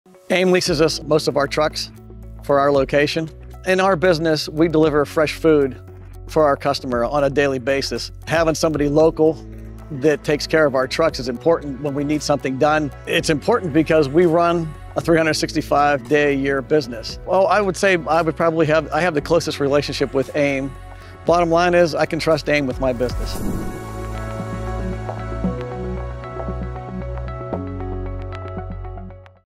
Leasing and The Bottom Line
Hear from Jim, an Aim Transportation Solutions customer on why he trusts Aim with his fleet.